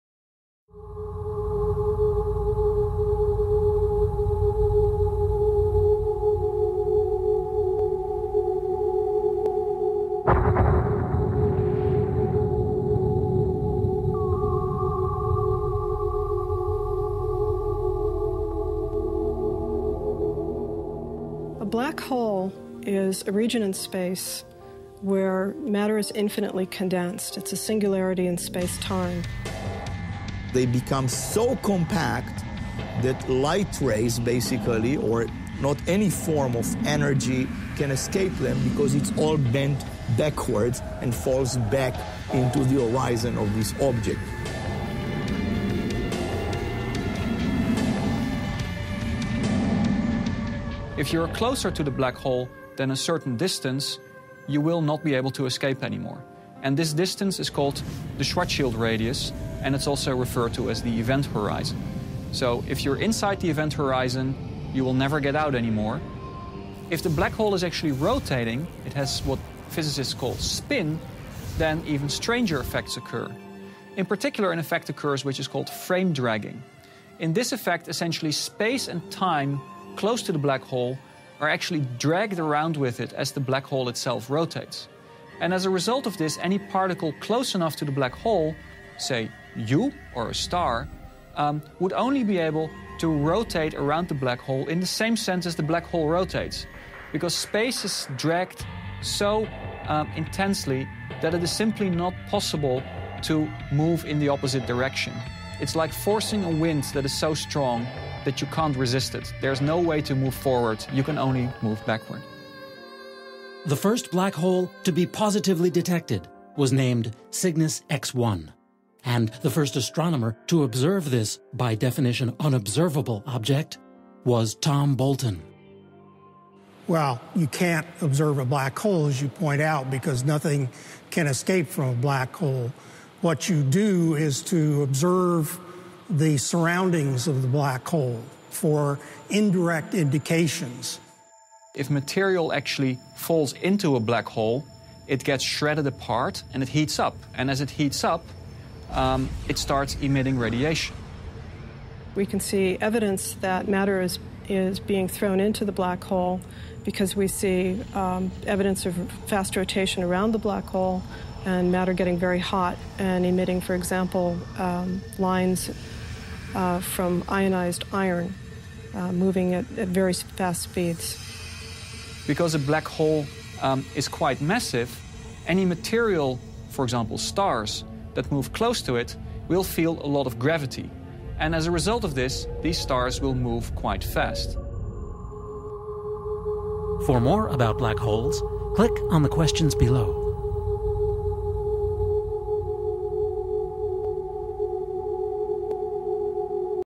Black Holes Warping Space & Time
World renowned experts explain how the fabric of space is altered near black holes; and what happens to normal matter (like you and me) if we stray too near to one. Also, the counter-intuitive nature of time near these radically strange objects.

Dave Brody